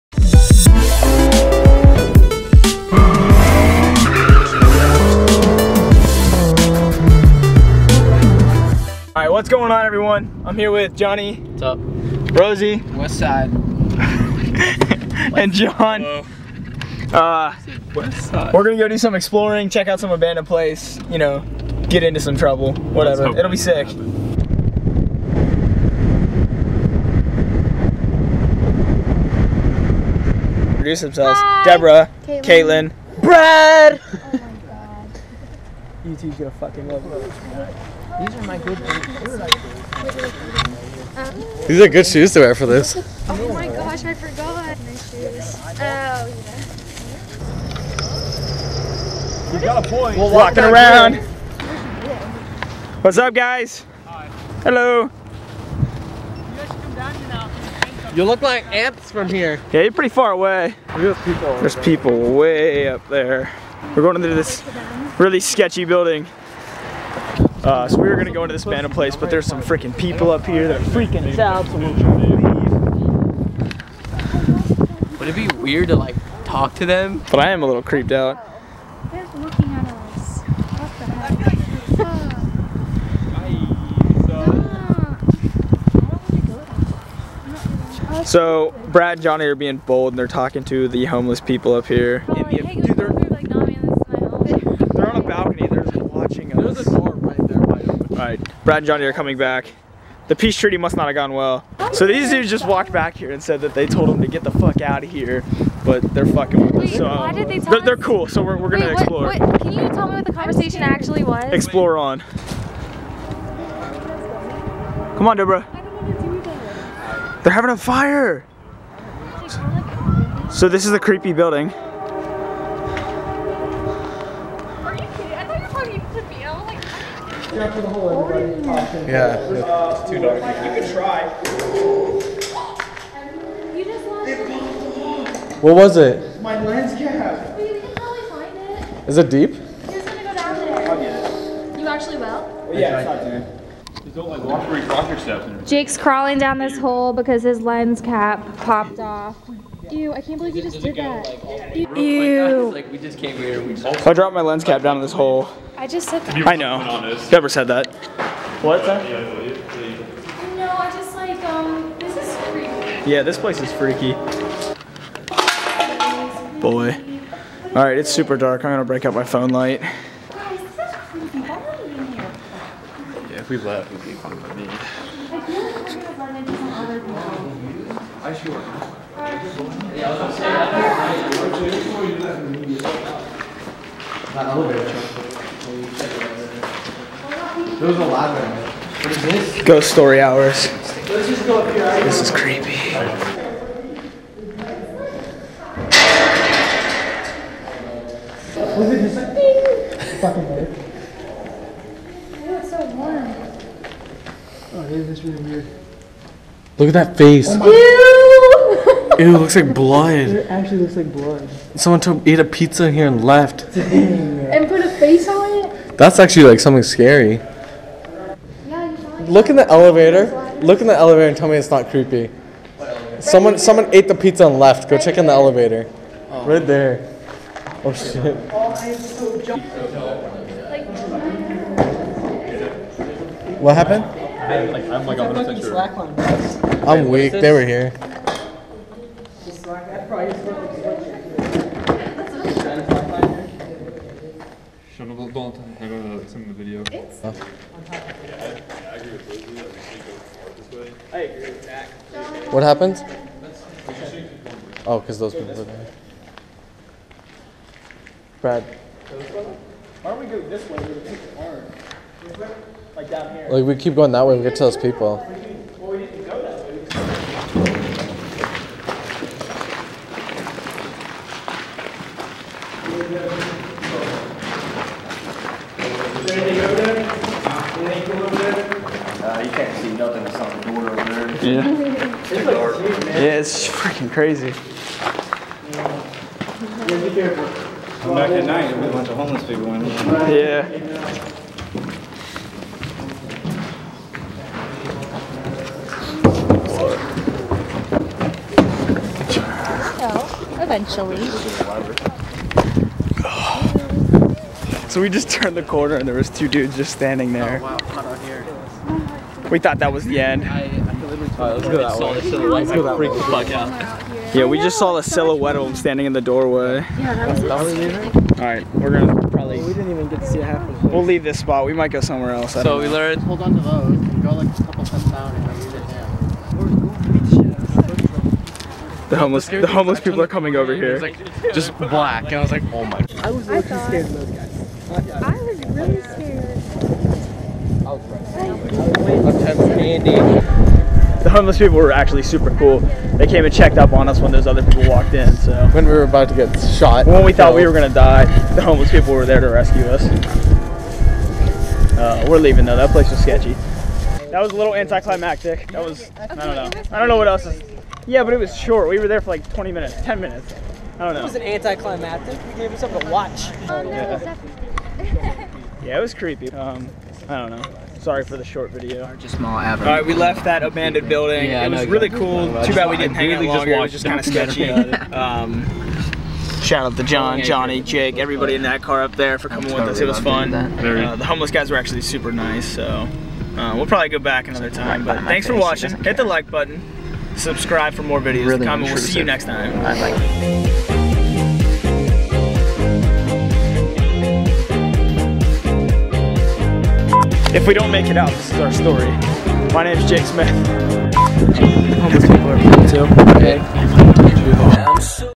SCARY ENCOUNTER IN ABANDON BUILDING
We went to explore an abandon building and ran into some people along the way.